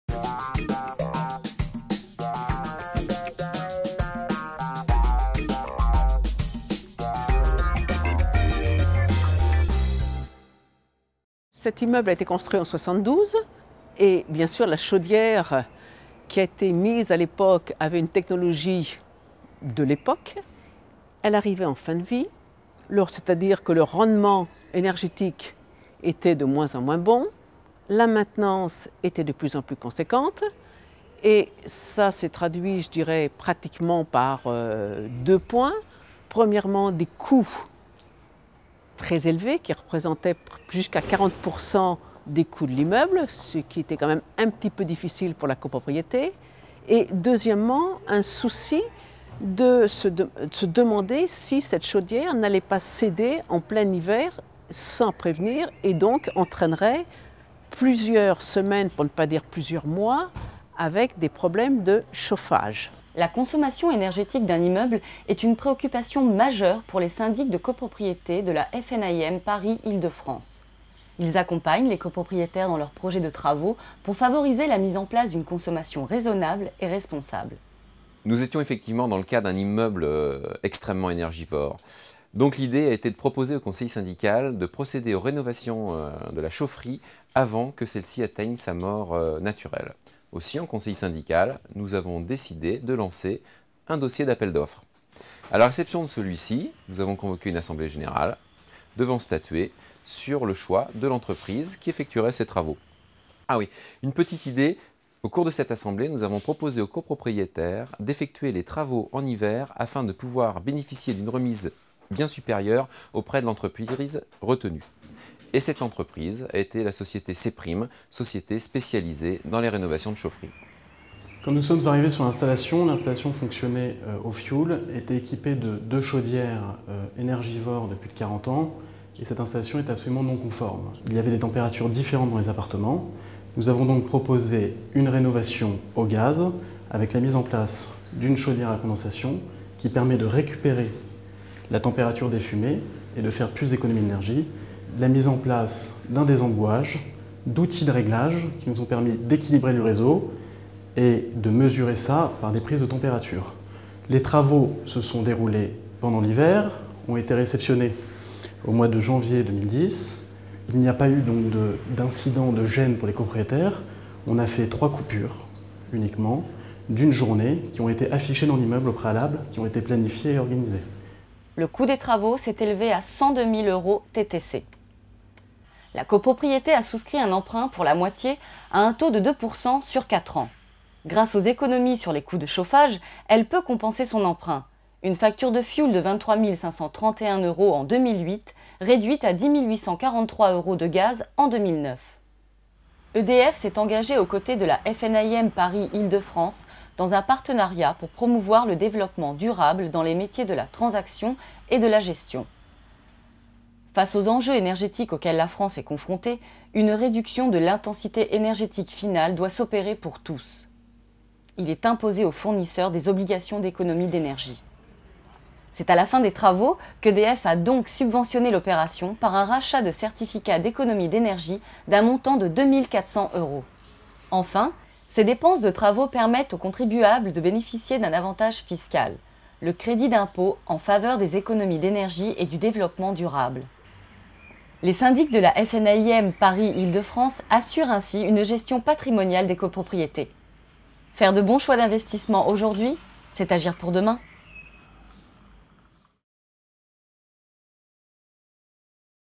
Faire des économies de chauffage en respectant l'environnement par le Syndic
D'hier à aujourd'hui n°4 : Faire des économies de chauffage en respectant l'environnement par le Syndic

Télévision immobilière du Grand Paris
FNAIM Paris Ile-de-France